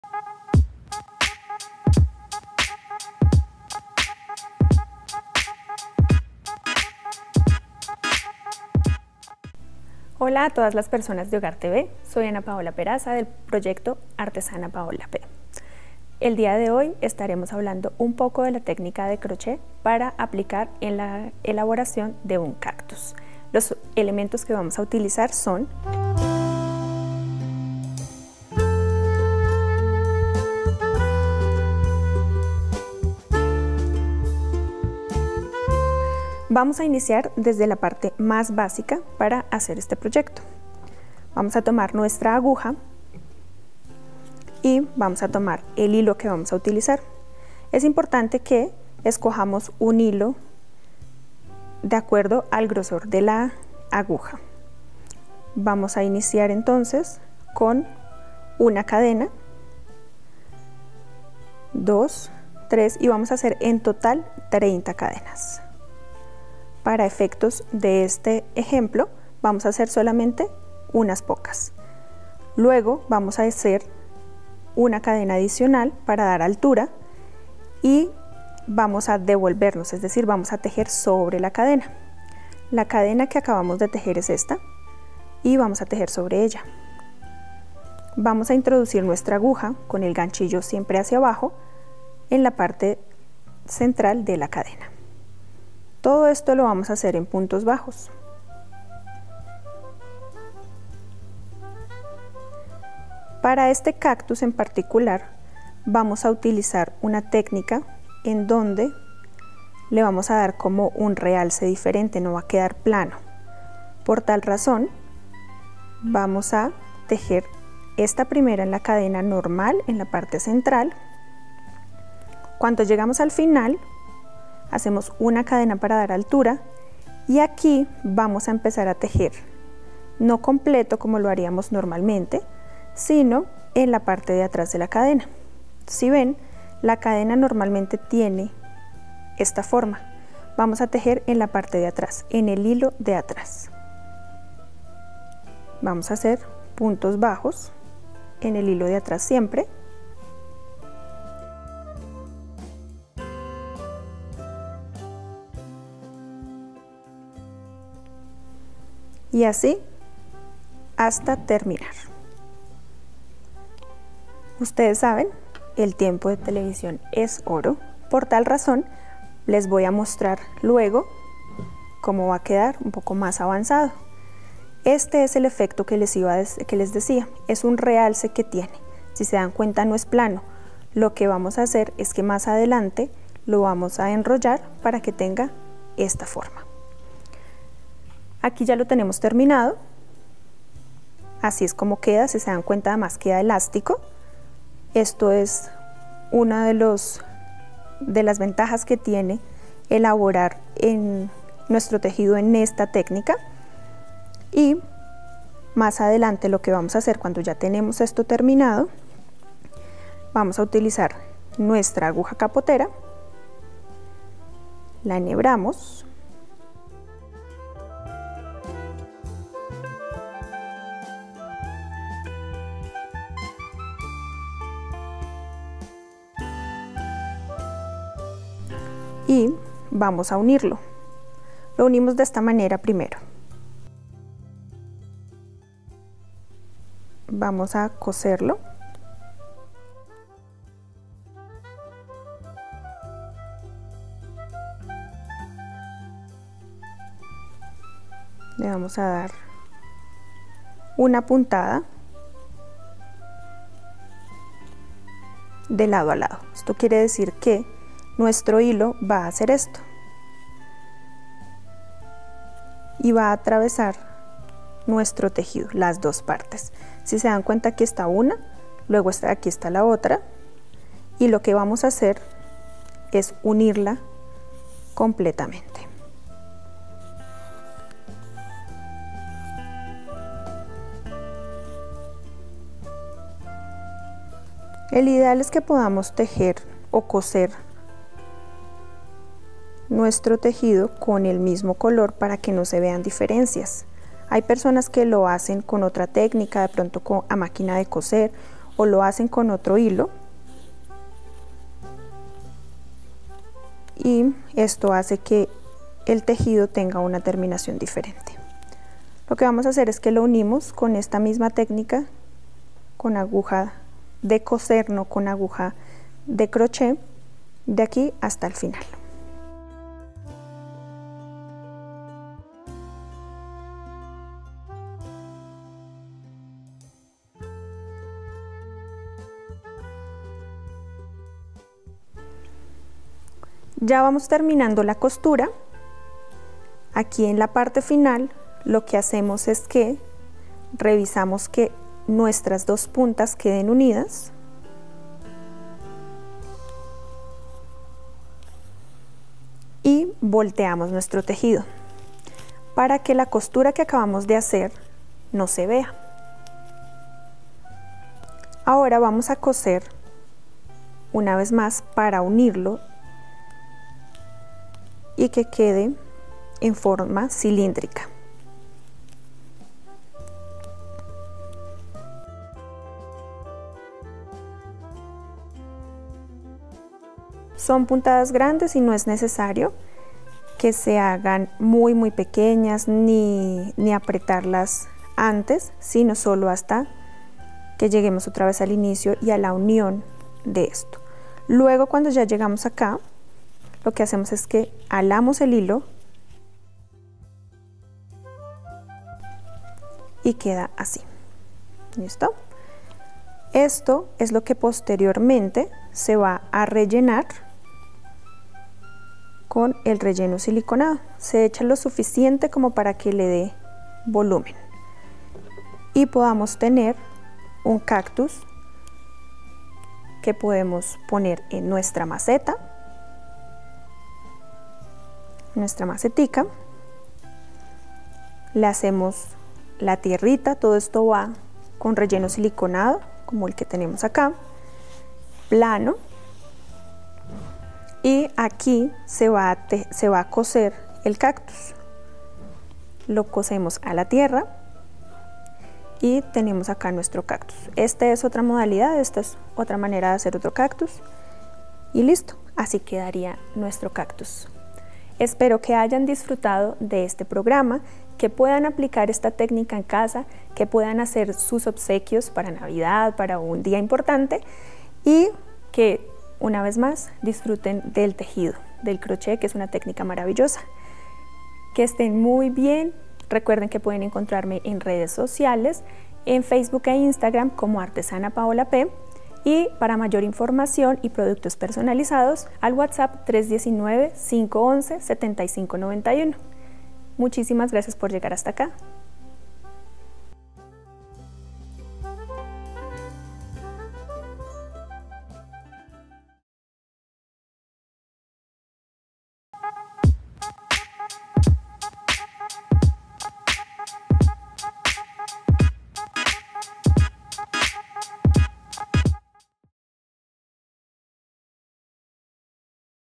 Como hacer un cactus de hilo tutorial de crochet - HogarTv producido por Juan Gonzalo Angel Restrepo
Los cactus son unas hermosas plantas, pertenecientes a nuestro planeta tierra y sobrevivientes de los desiertos. Los cactus son originarios de América. Podemos encontrar cactus silvestres desde Canadá hasta la Tierra del Fuego (Argentina).

Los Cactus y otras Suculentas representan un grupo de fascinantes plantas. Ellos han desarrollado un mecanismo de defensa ante el inclemente calor y las largas temporadas de sequía que en su lugar de vivienda se originan.

¡SÍGUENOS!

Invitado: